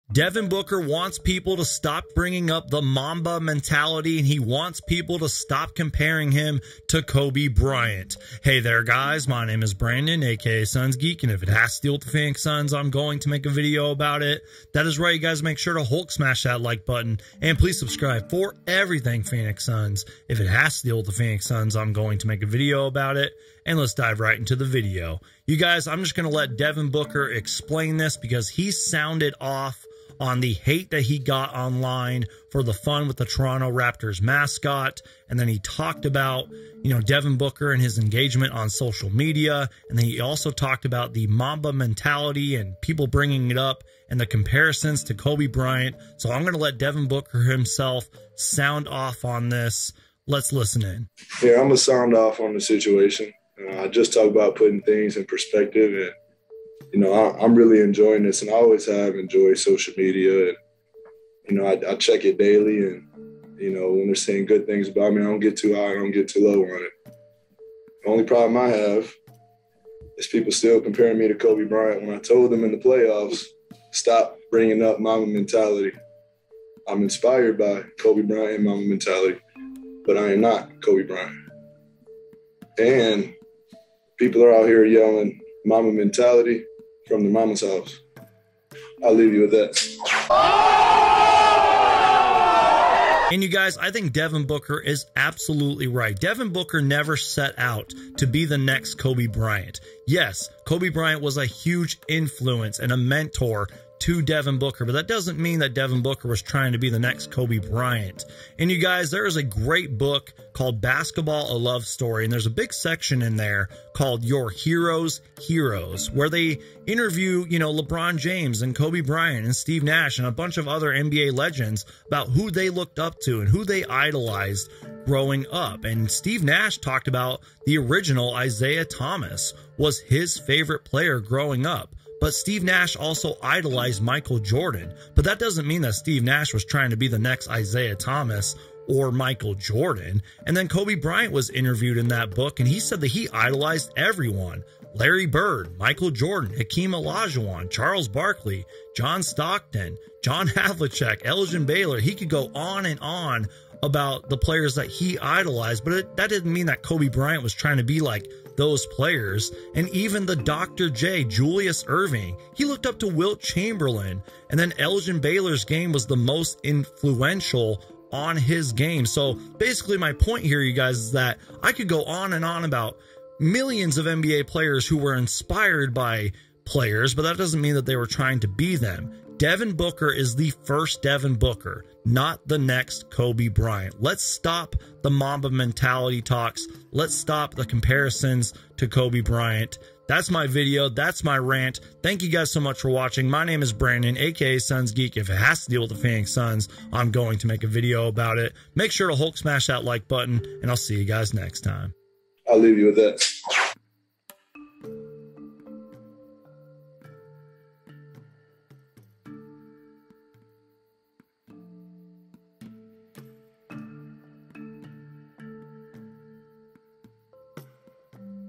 Devin Booker Sounds Off On Kobe Bryant Comparison & Mamba Mentality
Devin Booker sounds off on the Kobe Bryant comparison, Mamba Mentality, and social media. Here is my video on it.

I'm very active on my IG story! Follow for more content! @ sunsgeekonyoutube